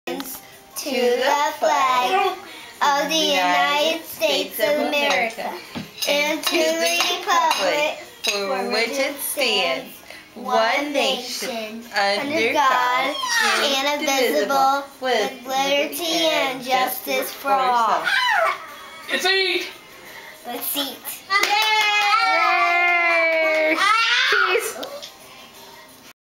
Pledge of Allegiance and Audrey
So, Audrey decided to say the pledge at our Thanksgiving dinner. Kinda Christmas Vacation like. Anyways, she had fun and was proud of herself.  :) Let's eat!